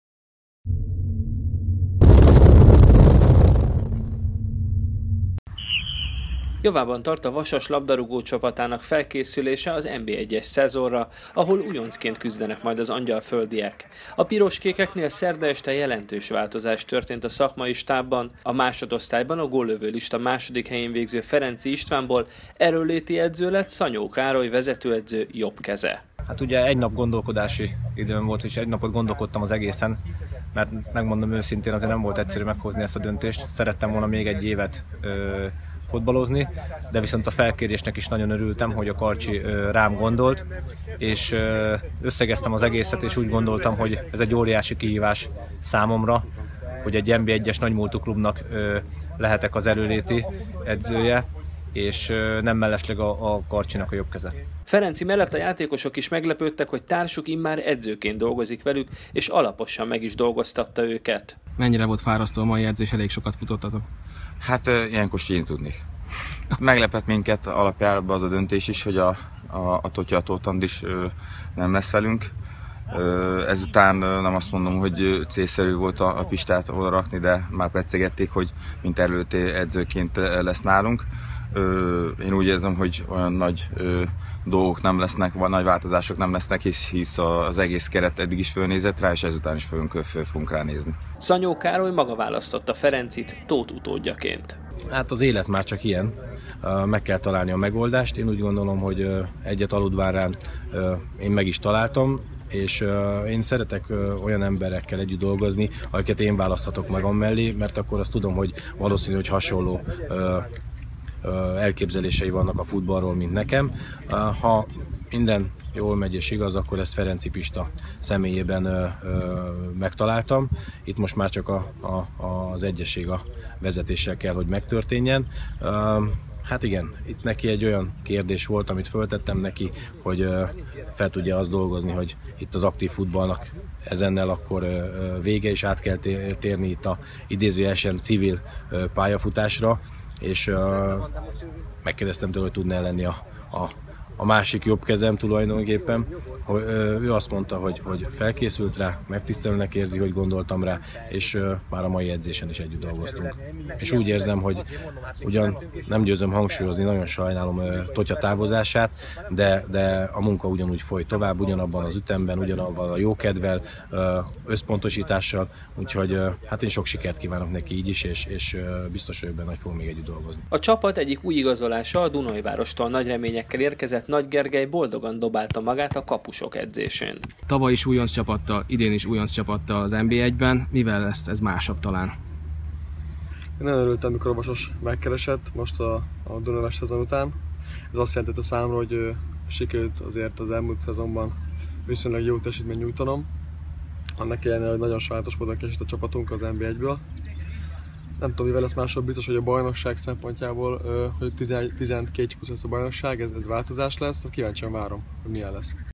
Új erőnléti edzőt kapott a Vasas
A korábbi erőnléti edző hirtelen távozása miatt Ferenczi István, korábbi játékos egy nap alatt kapott egy teljesen új megbízást.